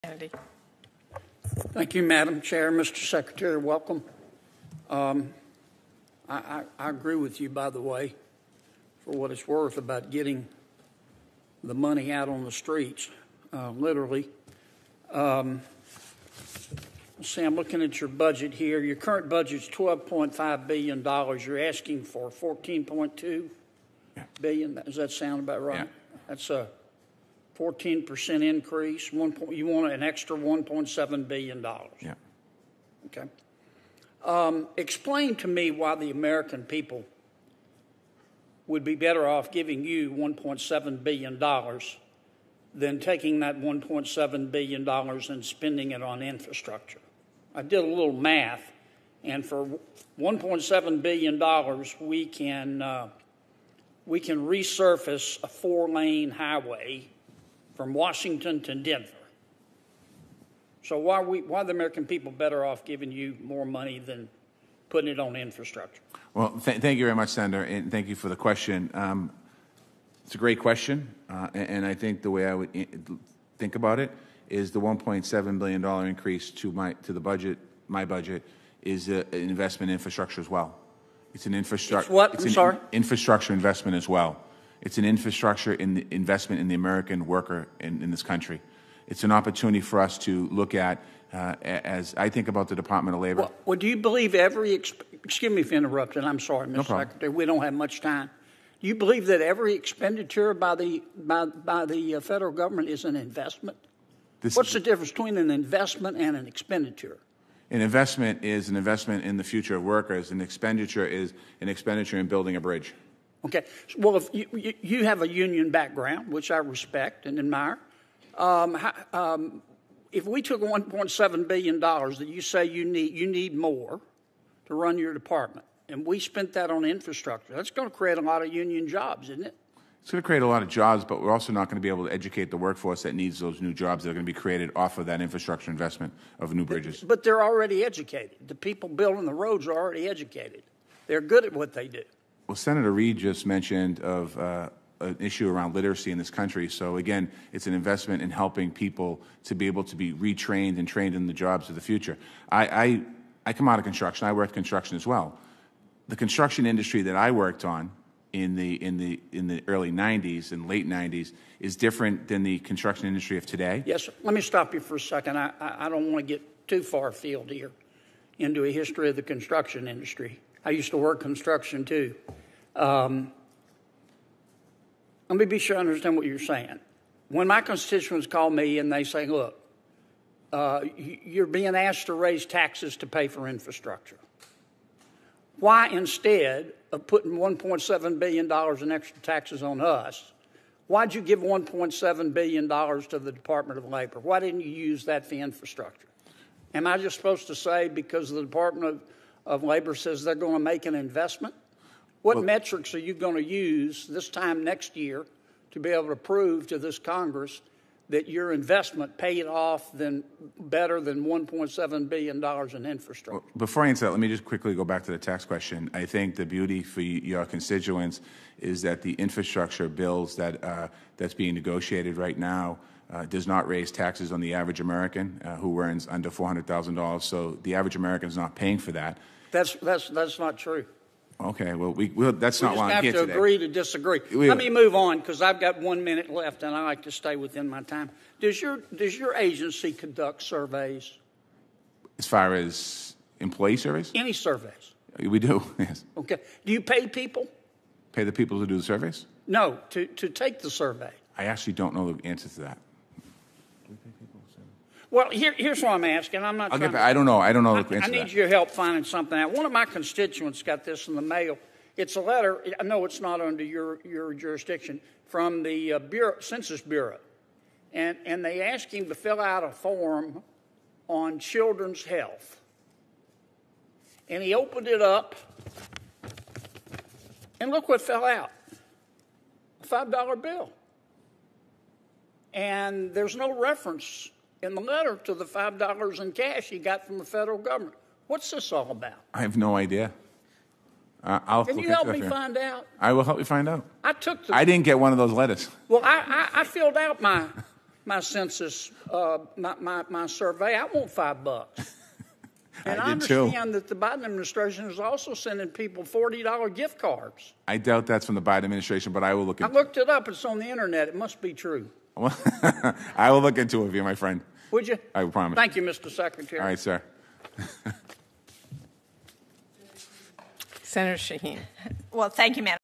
'One Of My Constituents Got This In The Mail': John Kennedy Presents Surprising Letter At Hearing
At yesterday's Senate Appropriations Committee hearing, Sen. John Kennedy (R-LA) questioned Labor Secretary Marty Walsh about a letter a constituent allegedly received about the census which contained a $5 bill.